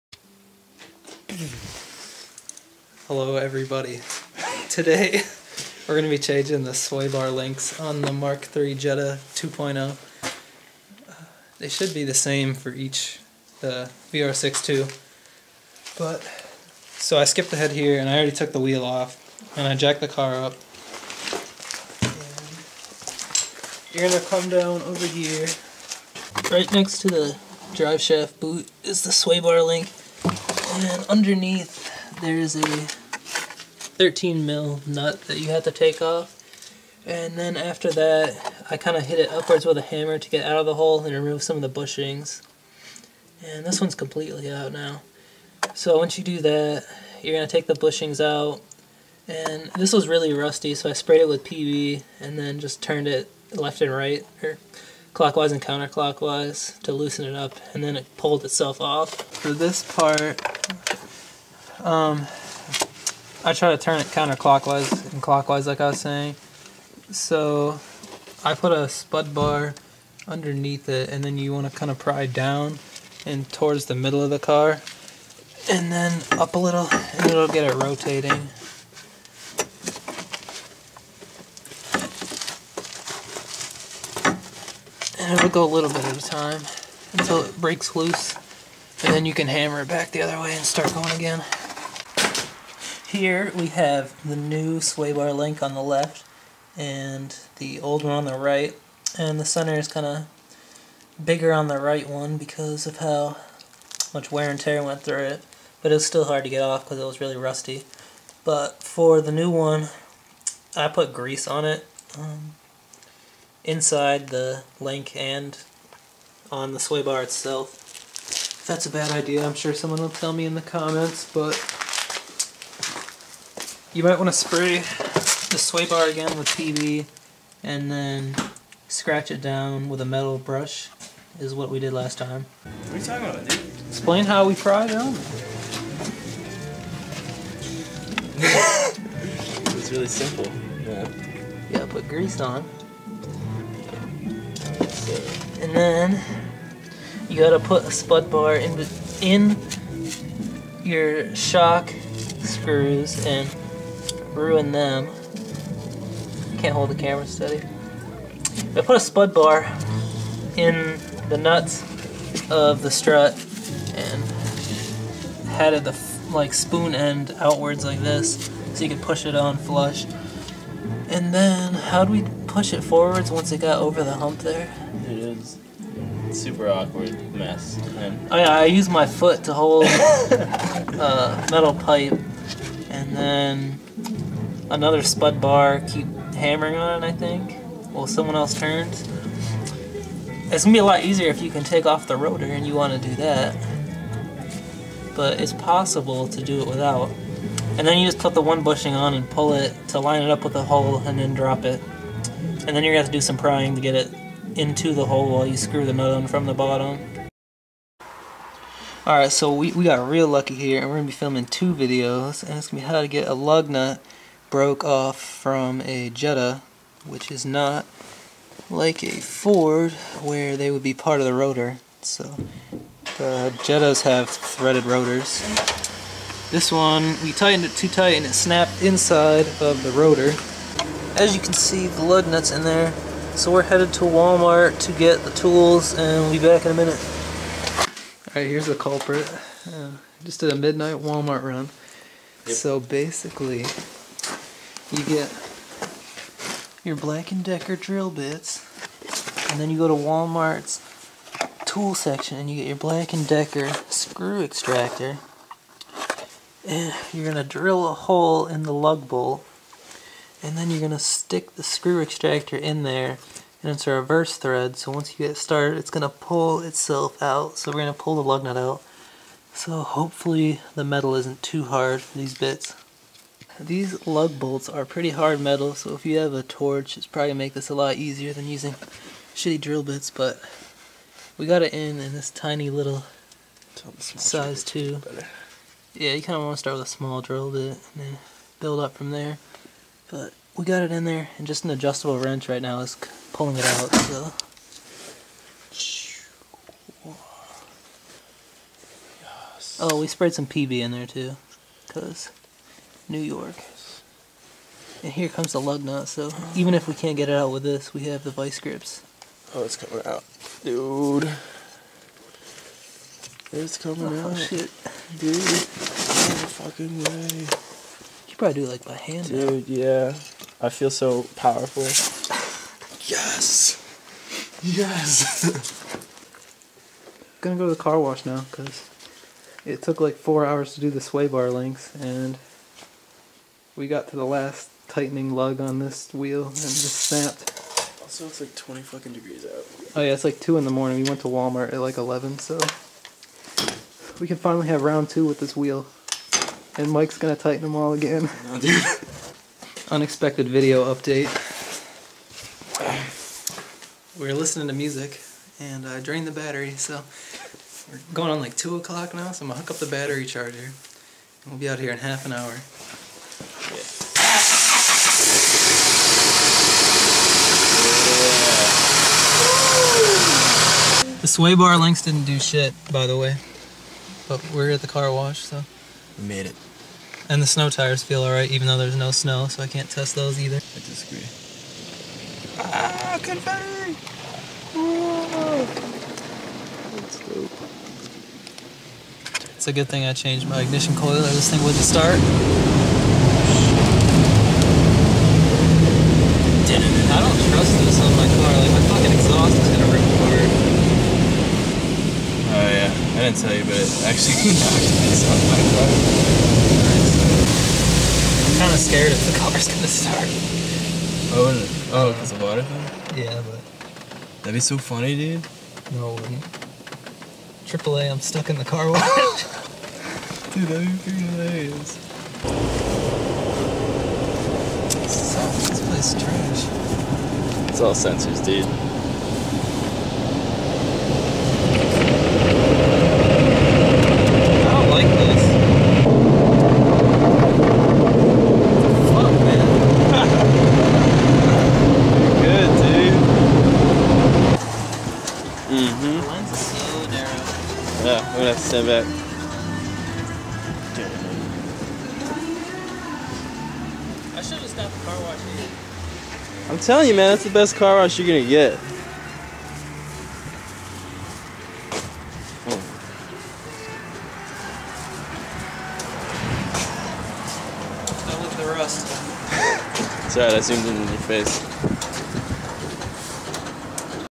Replace Sway Bar Links & Remove A Broken Lug Bolt From A Mk3 2 0L Jetta
Watch Mike and I try to work on my car and get it to the car wash. I switched from using my phone back to my Sony Handycam, so it felt a little unfamiliar and some shots are shaky.